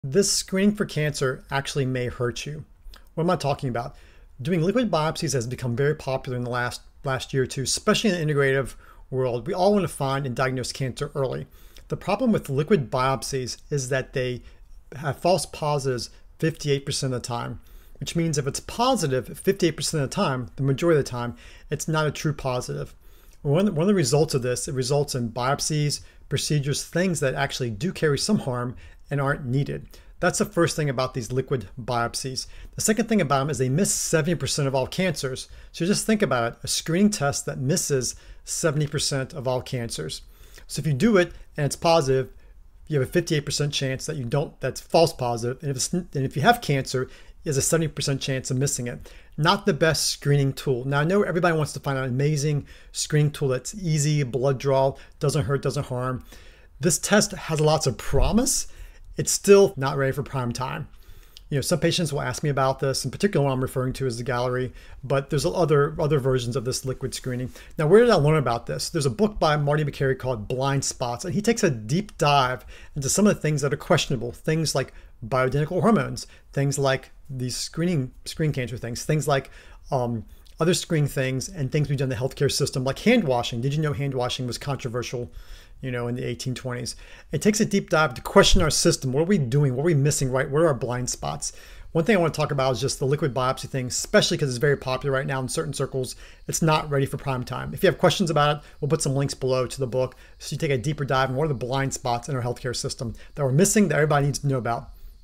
This Screening for Cancer Actually May Hurt You
Today I wanted to talk through the controversial world of liquid biopsies for cancer screening. While these tests have gained popularity in the integrative health community for their promise of early cancer detection, they come with significant drawbacks. Liquid biopsies can have a staggering 58% false positive rate, meaning most positive results may not indicate actual cancer. Even more concerning, these tests miss approximately 70% of all cancer cases, raising serious questions about their reliability as a screening tool.

Drawing insights from Marty Makary's book, Blind Spots, we explore the larger issues within our healthcare system, including questionable practices and overlooked challenges. This video is essential for anyone curious about cancer screenings and the potential pitfalls of emerging medical technologies.

Join us as we unpack these important topics and highlight the need for critical examination in healthcare.